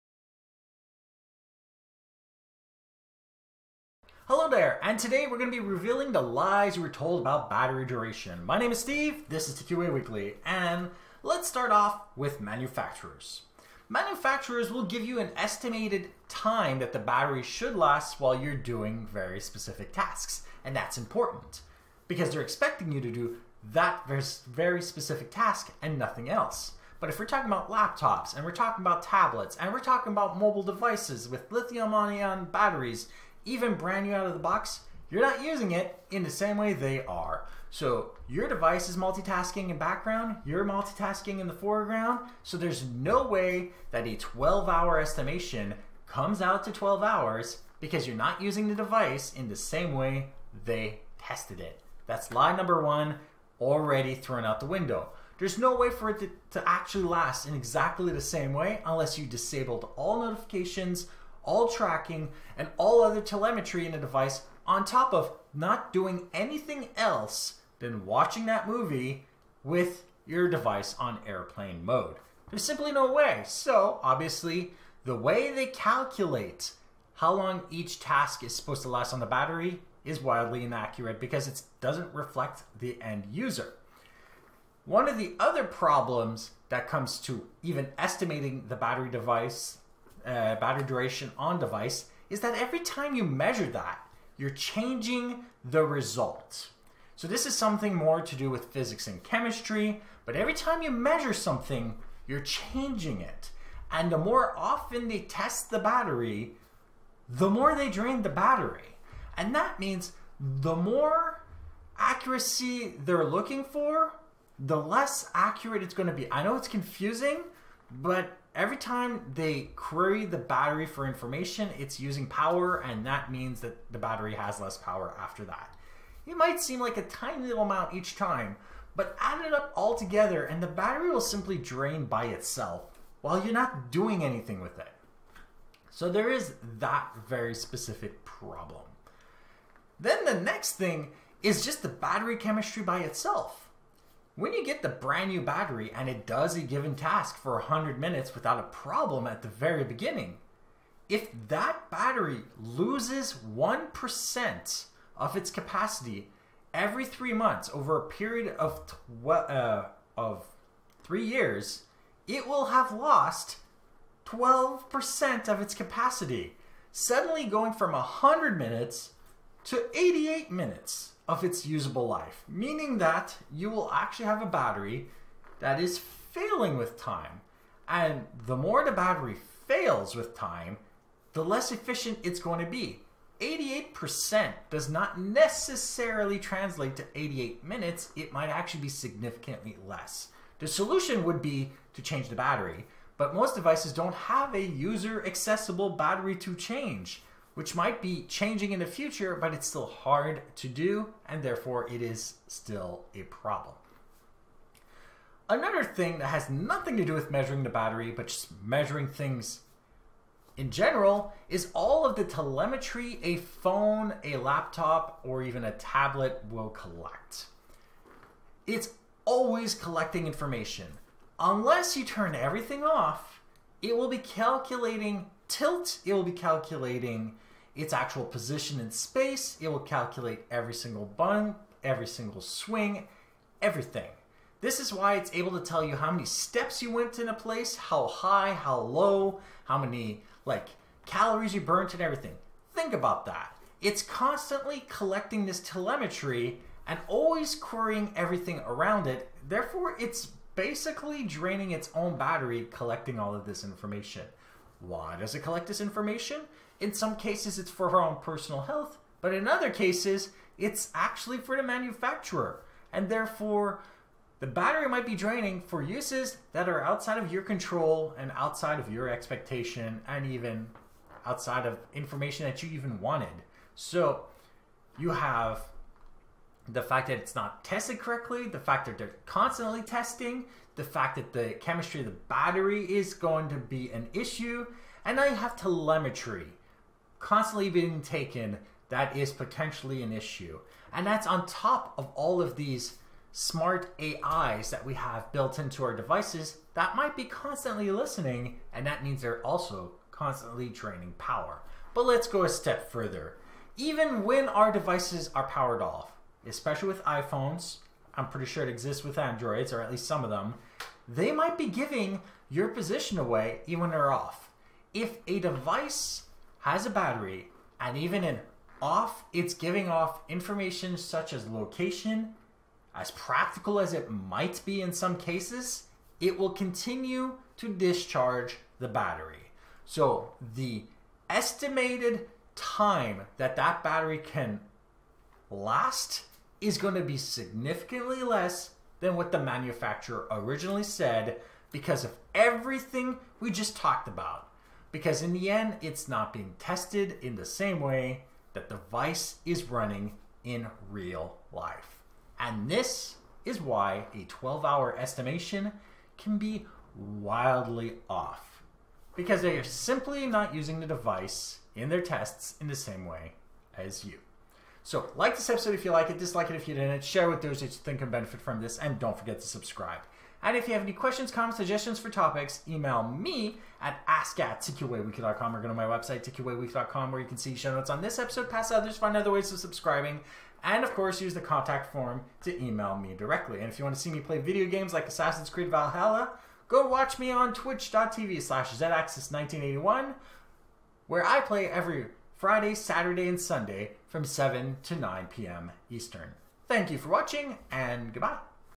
The Lies You Are Told About Battery Duration Revealed!
When purchasing a device with a lithium-ion battery, you get an expected usage time estimation that is rarely ever met, what is going on?